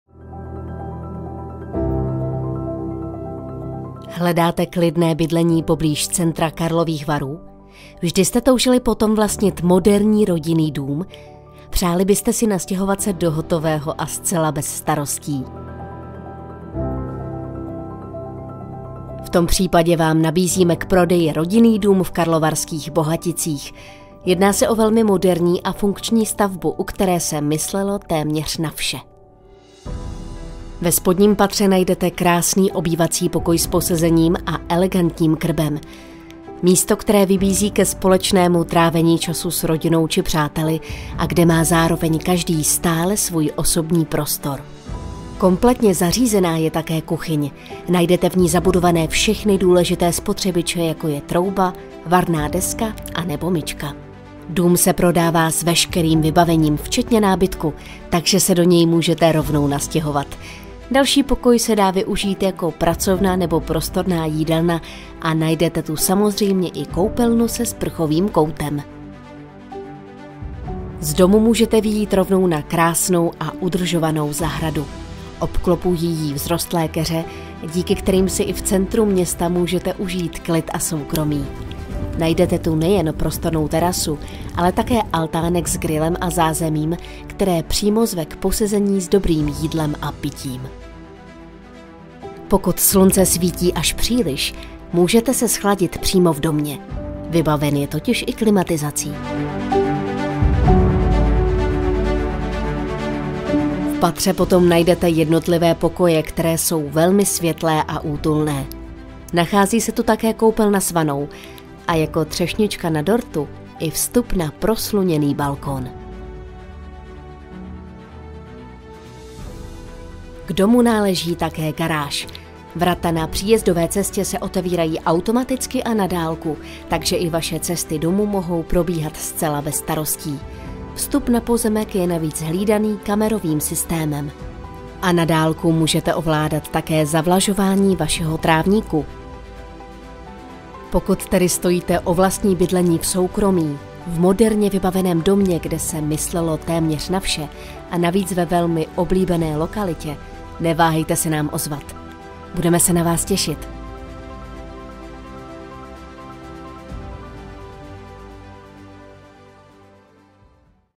Rodinný dům na prodej v Karlových Varech s nádhernou zahradou a altánem
Rodinný dům v Karlových Varech s nádhernou zahradou a altánem.